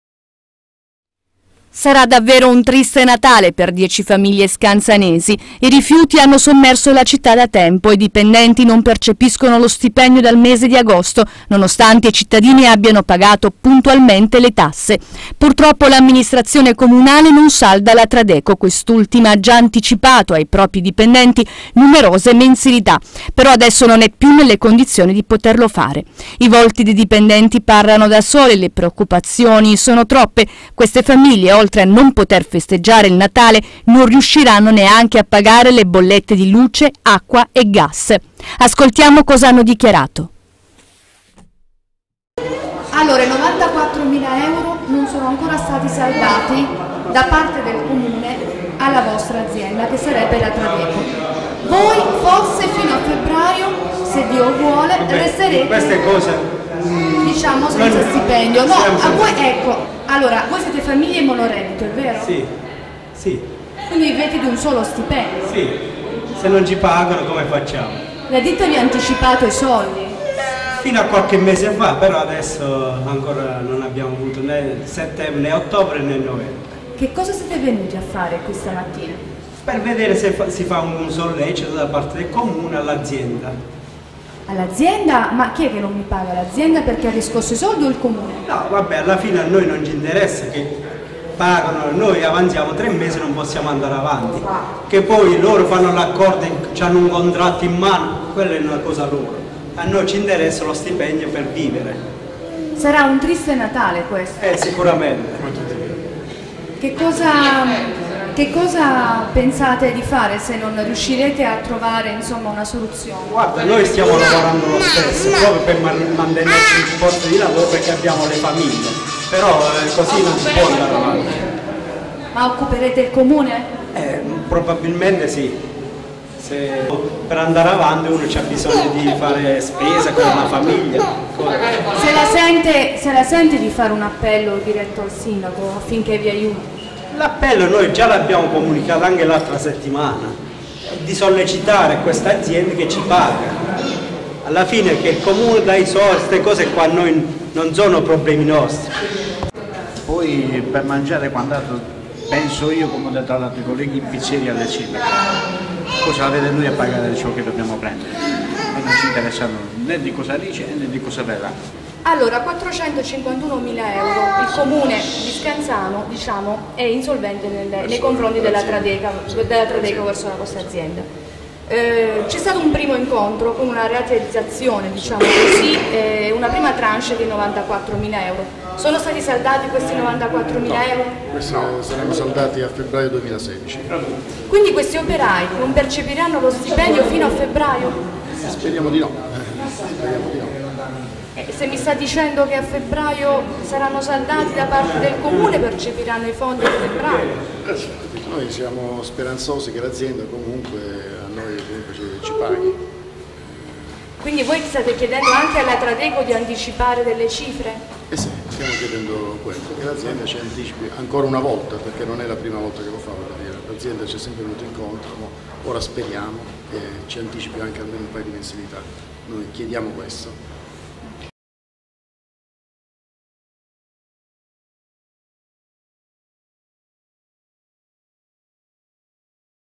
BLU TV - RIFIUTI A SCANZANO J.CO, OCCUPATO IL COMUNE
Natale triste per 10 famiglie Scanzanesi che non percepiscono lo stipendio dal mese di agosto e la Ditta Tradeco che a sua volta non viene pagata dal Comune di Scanzano ha anticipato ai dipendenti numerose mensilità.
di Mary PADULA